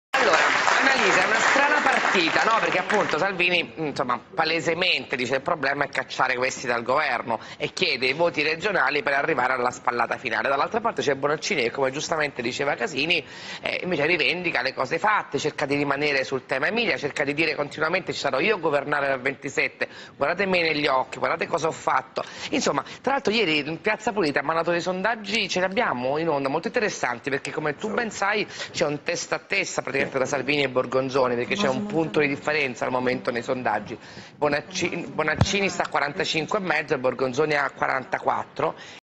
lasciatelepoltrone
Nell’Emilia Romagna “ROSSA”, Lucia Borgonzoni guadagna terreno. La sinistra trema, il M5S finisce la sua avventura politica.